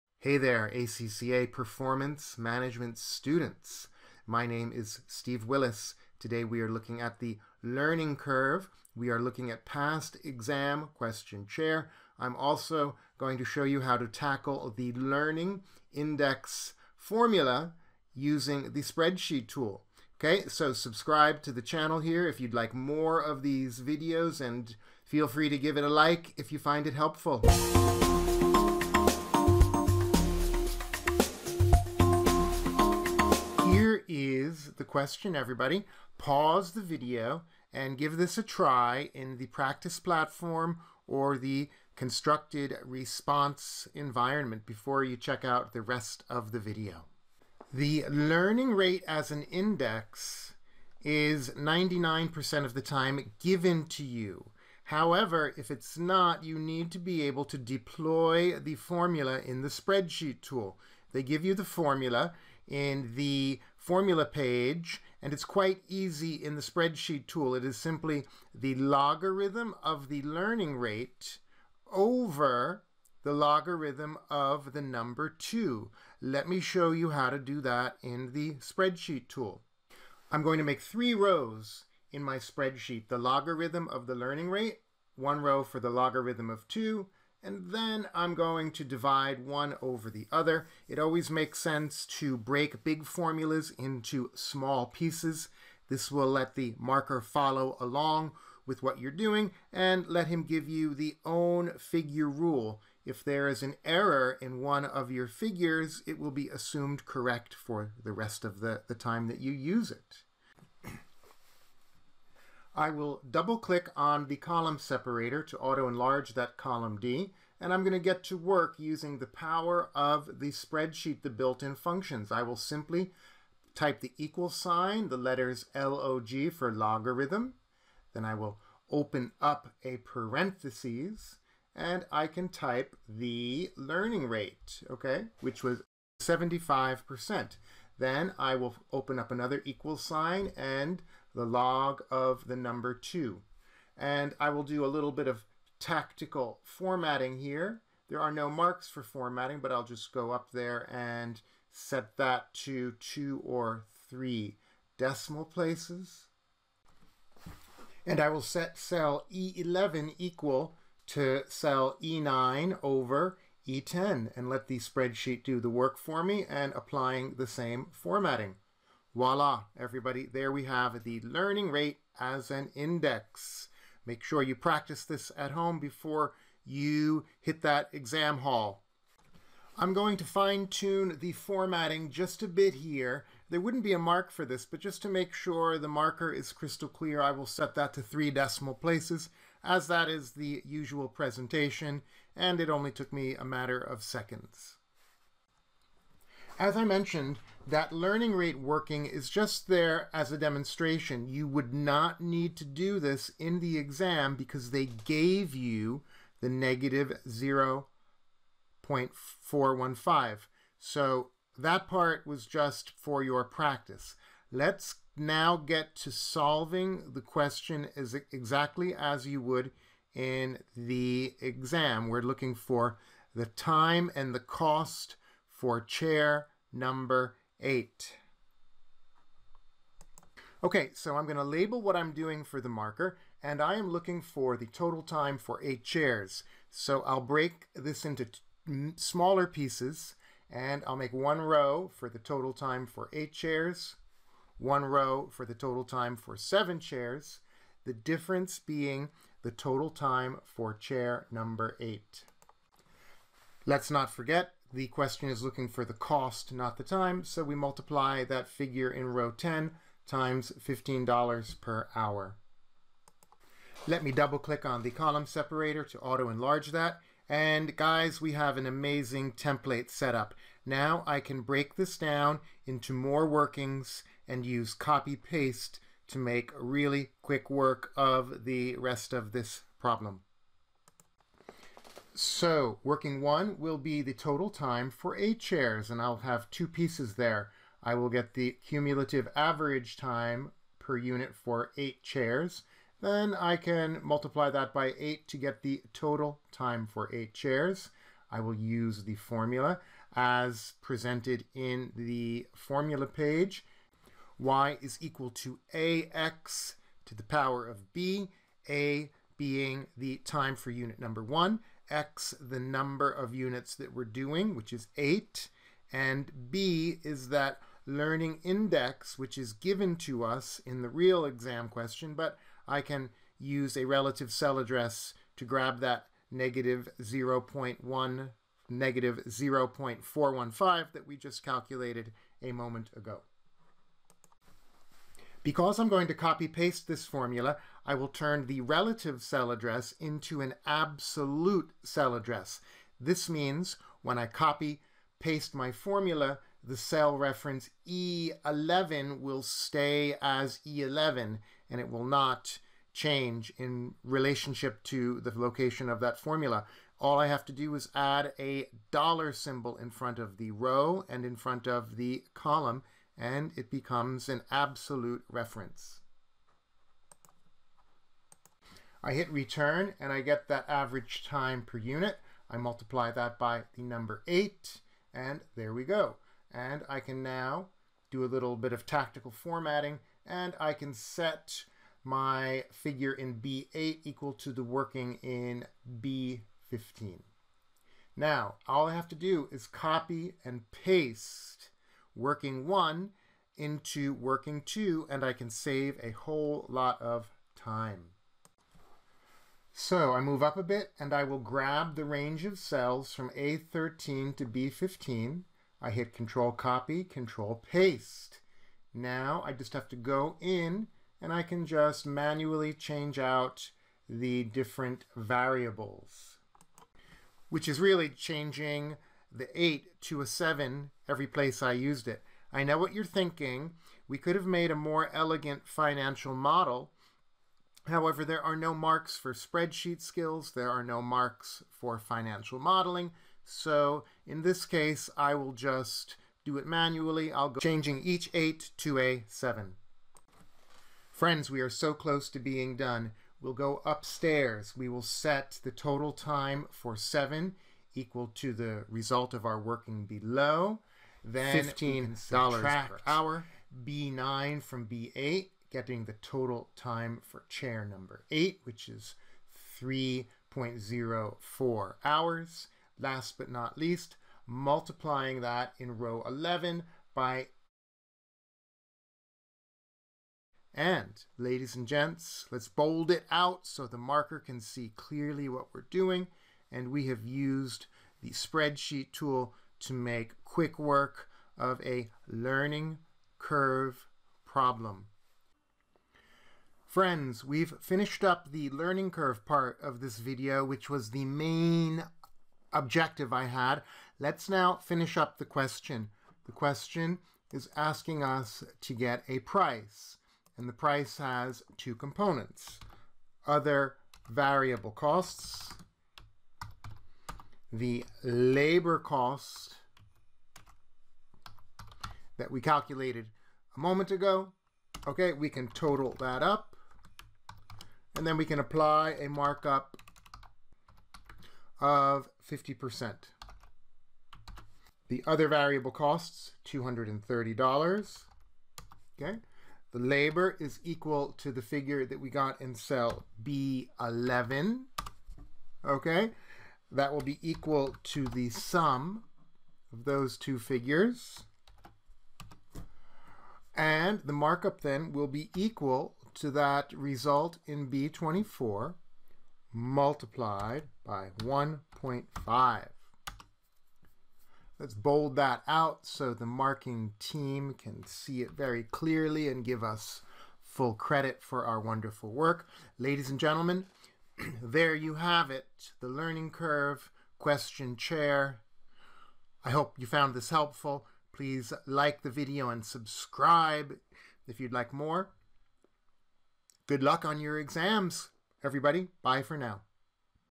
How to Calculate a Learning Curve | ACCA CBE PM question Chair | learning Index help (LogLR/Log 2)
CBE & spreadsheet exam technique, help, and tips for ACCA students sitting paper PM / F5, question Chair.
Learn how to calculate time-per-unit when a learning curve is in effect using the ACCA spreadsheet tool in CBE (computer based exams). This exam technique is a must-know for ACCA PM (Performance Management--old F5) CBE students. You'll see it's much faster and easier to do with the spreadsheet, rather than your calculator.  I'll also show you how to calculate the learning Index, using the formula (LogLR/Log 2).
This video takes you through Part A of Question Chair, from the 2016 PM Specimen Exam.
This video shows you how to pass your ACCA CBE exams.

Learning rate as an index explained at 00:45

Steve Willis ACCA
Helping you pass your ACCA exams